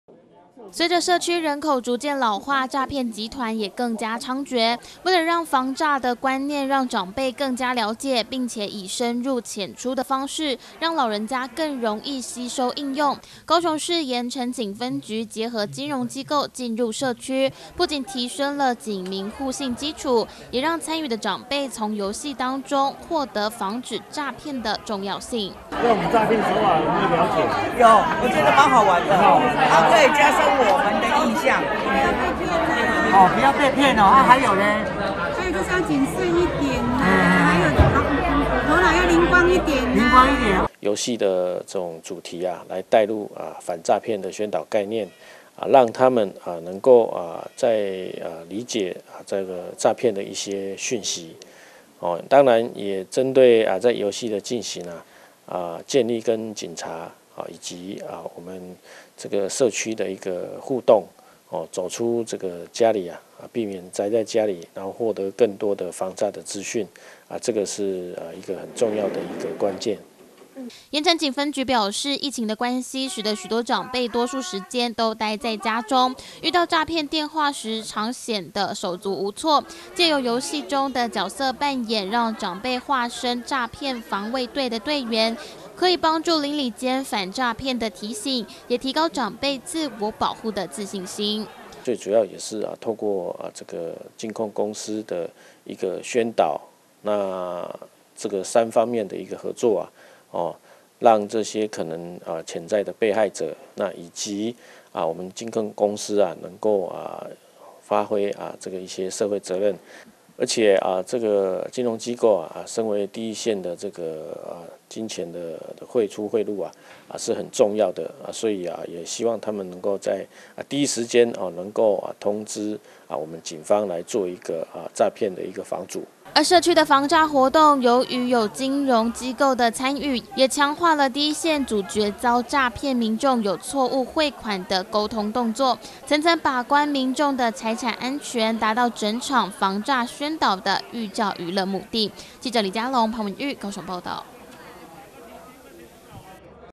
樂齡防詐宣導|齊防詐警民互動 結合桌遊長輩輕鬆理解
【港都新聞】齊防詐警民互動 結合桌遊長輩輕鬆理解
詐騙近來的猖獗，長輩該如何了解並且保護自己的身家財產，日前高雄市鹽埕分局結合金融機構深入社區，以最夯的桌遊遊戲帶入防詐觀念，讓長輩們玩得不亦樂乎外，也提昇了警民互動，潛移默化得讓參與民眾了解到防詐的重要性。